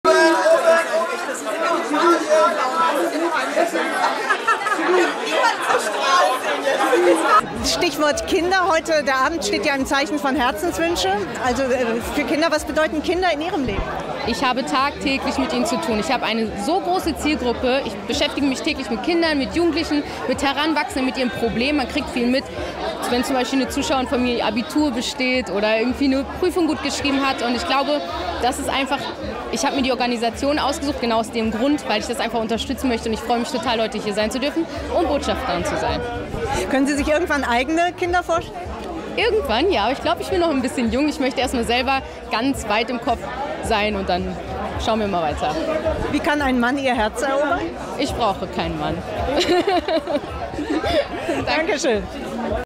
Shirin David: "Ich brauche keinen Mann!"
Sie sah mal wieder umwerfend aus! Vor wenigen Tagen trafen wir Shirin David beim Raffaello Summer Day. Mit uns sprach sie über ihr Engagement für den guten Zweck, Kinder und Männer...